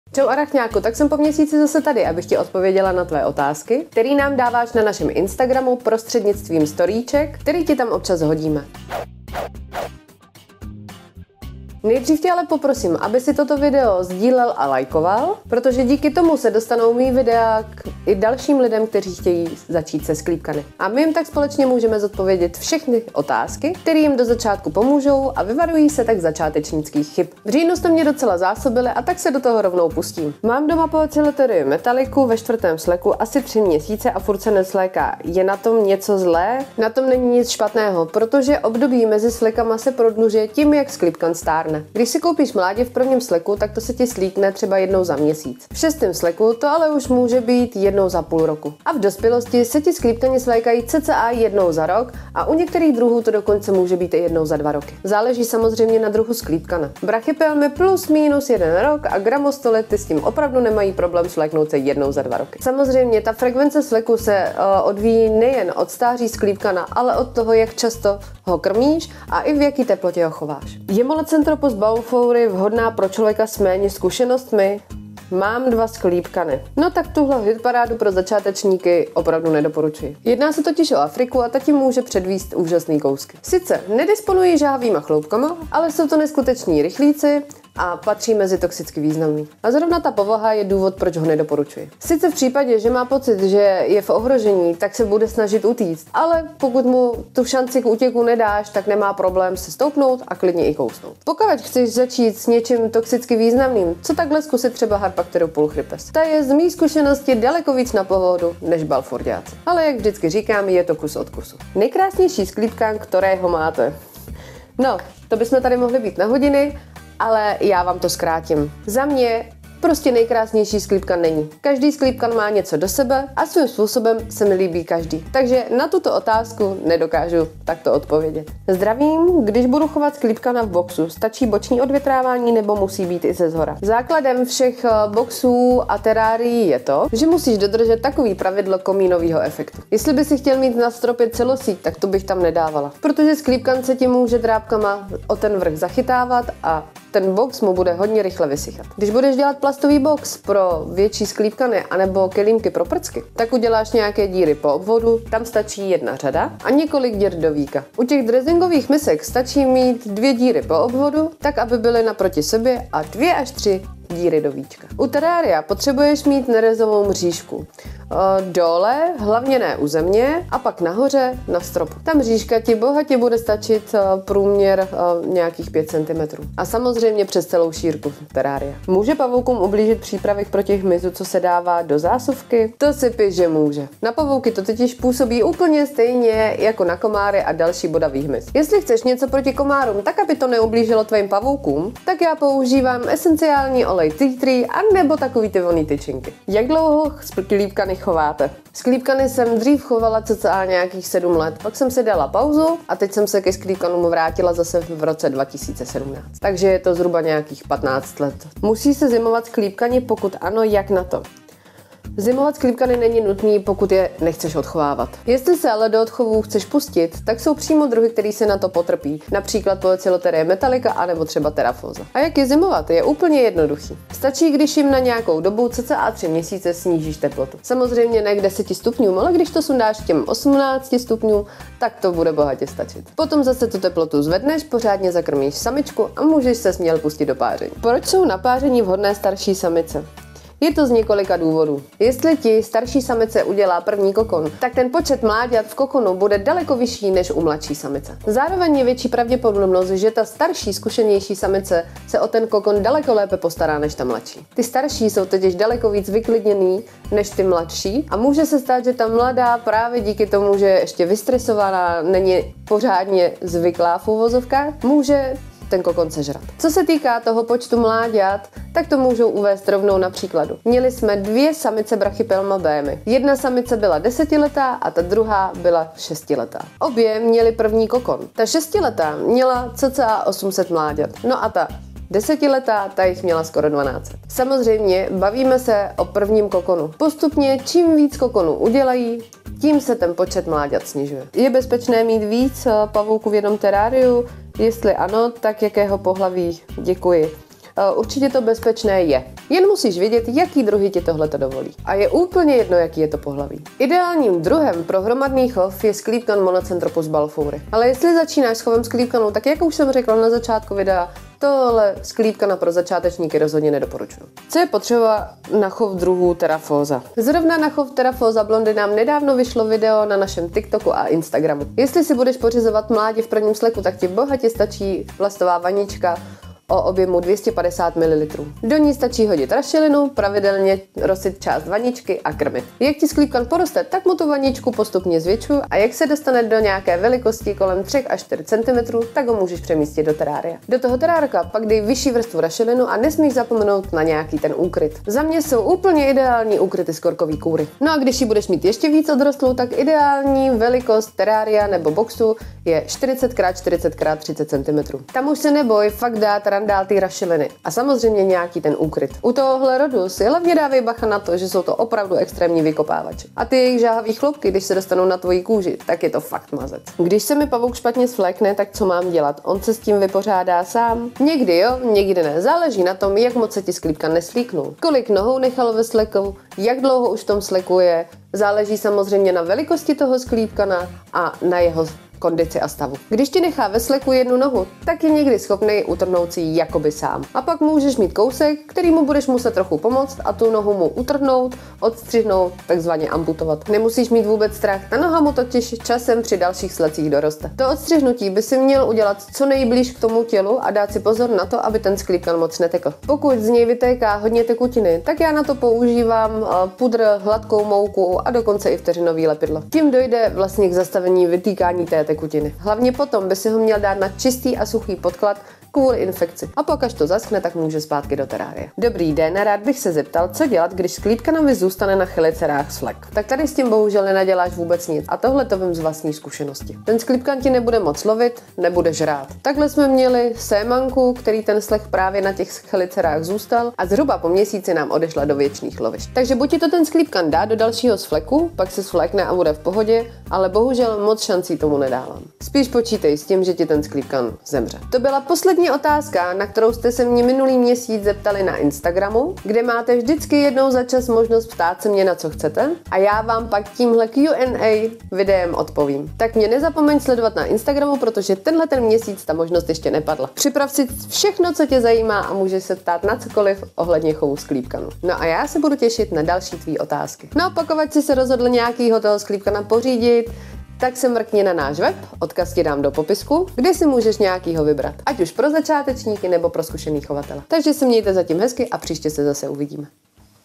Může sklípkanovi ublížit přípravek na hmyz? Musí se zimovat sklípkani? (Q&A #2)
Pokračujeme dalšíma otázka, který jste nám napsali na našem Instagramu. Proč pářit starší samičky? Co je potřeba pro chov sklípkanů rodu Theraphosa? Co dělat když se pavouk špatně svlékne nebo mu zůstane svlek na chelicerách?

Tyhle a další otázky si odpovíme v dnešním videu.

00:30 Mám doma Poecilotheria metallica ve 4. svleku, asi 3 měsíce a furt se nesvlékla, je na tom něco zlé?
01:10 Je Monocentropus bafouri vhodná pro člověka s méně zkušenostmi?
01:51 Nejkrásnější sklípkan, kterýho máte?
02:08 Když budu chovat sklípkana v boxu. Stačí boční odvětrávání neo musí být i shora?
02:58 Může pavoukům ublížit přípravek proti hmyzu, co se dává do zásuvky?
03:16 Jak dlouho sklípkany chováte?
03:28 Musí se zimovat sklípkani? Pokud ano, jak na to?
03:57 Proč jsou na páření vhodnější starší samice?
04:51 Je bezpečné mať viac vtáčkarov v jednom teráriu? Ak ano tak akého pohlavia?
05:14 Co je potřeba na chov druhů Theraphosa
06:06 Když se mi pavou špatně svlékne, tak co mám dělat? On se s tím vypořádá sám?
06:58 Co dělat, když sklípkanovi zůstane na chelicerách svlek?